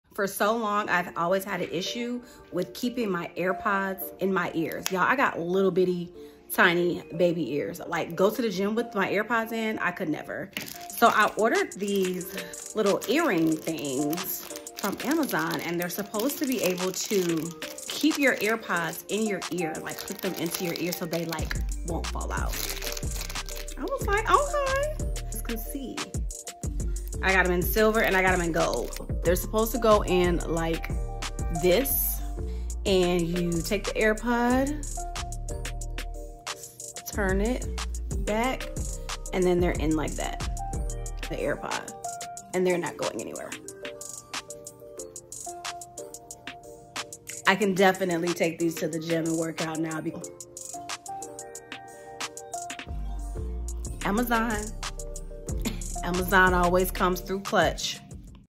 NEVER LOSE YOUR AIRPODS AGAIN!!!
It’s so annoying when your AirPods falls out your ear constantly!!! These earrings are a game changer and you will never lose an AirPod again!!! I got them from Amazon and they are in my storefront.

Here is my website. Click the link that says “Amazon Storefront” to find the AirPod earring holder